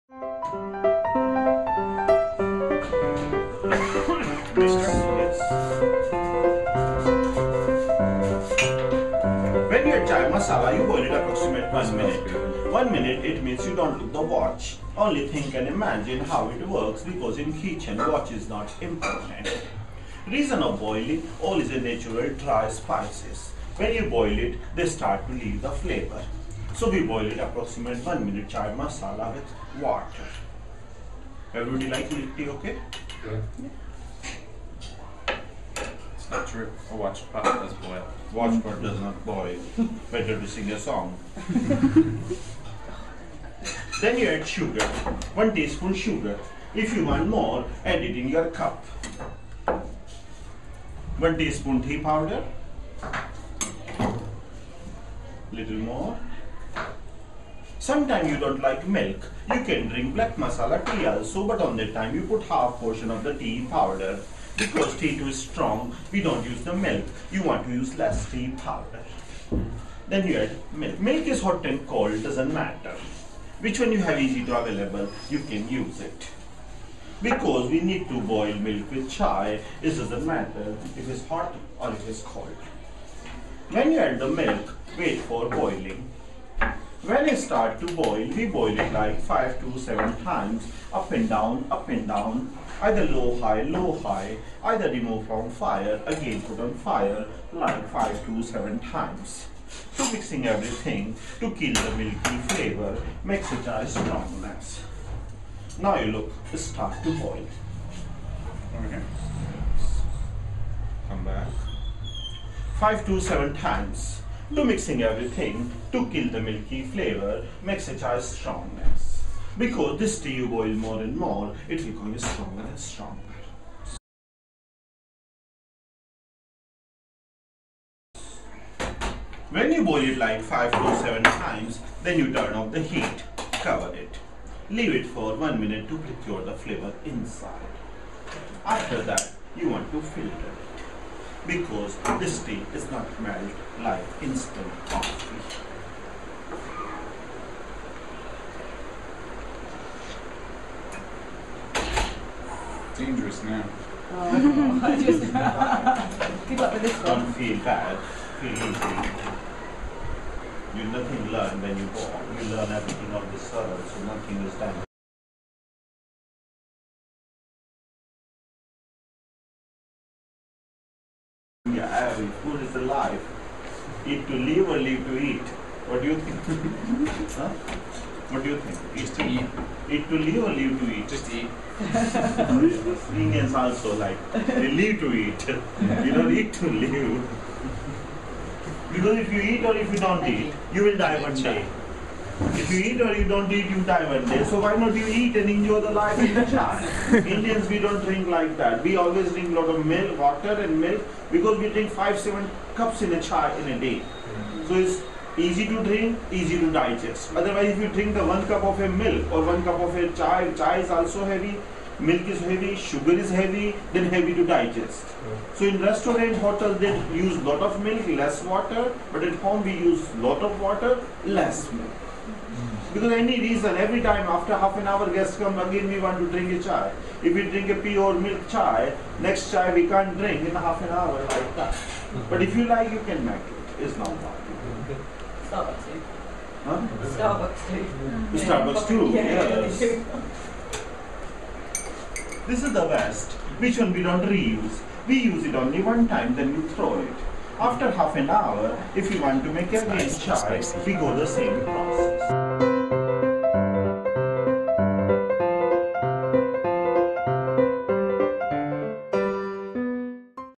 How to Make Masala Chai
In Udaipur, India, we went to the Spice Box for cooking classes.  In this segment, our teacher shows Jonathan how to make Indian Masala Chai.  By the way, "masala" is just the term for "spice mix".  There are a huge number of masalas in India - and basically anyone can invent a new masala by mixing together different spices into a new spice mix.

We love Indian masala Chai.